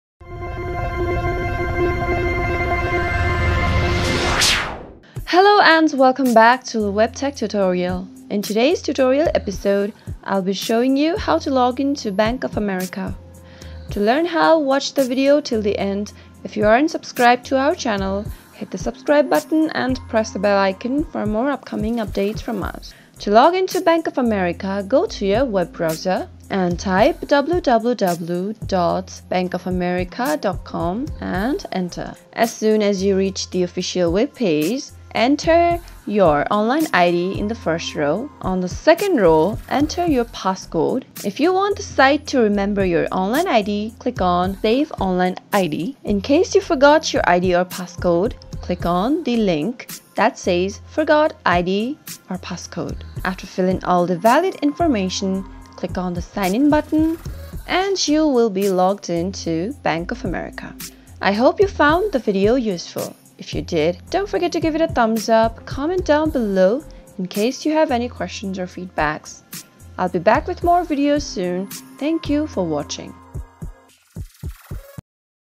Bank of America Login 2020 (Desktop) | Bank Of America Sign In Online Tutorial
The Bank of America Corporation is an American multinational investment bank and financial services holding company. Bank of America is one of the Big Four banking institutions of the United States.

In this video tutorial, we will teach you how to login to your Bank of America account online using your Bank of America online ID and passcode in less than two minutes' time.

Make sure to watch this video until the end and follow the stepwise procedure and you'll be able to sign in to your Bank of America account easily.